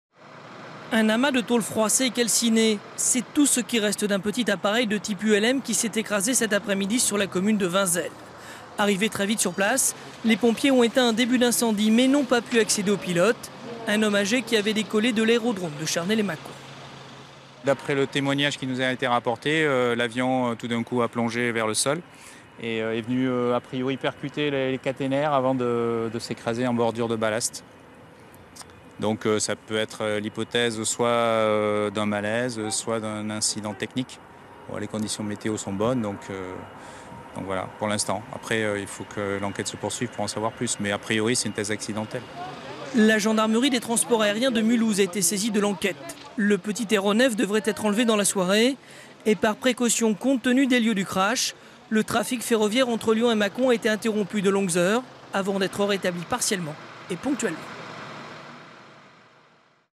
Vinzelles : Un accident d'avion fait un mort en s'écrasant près de la ligne SNCF
Vendredi 7 octobre vers 15h30, un petit avion de type Zehner s'est écrasé entre la commune de Vinzelles et de Varennes-lès-Mâcon (Saône-et-Loire) à proximité de la ligne SNCF Mâcon-Lyon. Le trafic ferroviaire a été interrompu dans les deux sens. Le bilan est de un mort.

Reportage de Damien Boutillet et Romy-Ho-A-Chuck
Avec
Colonel David Rey, commandant du groupement de gendarmerie de Saône-et-Loire
Plus d'informations :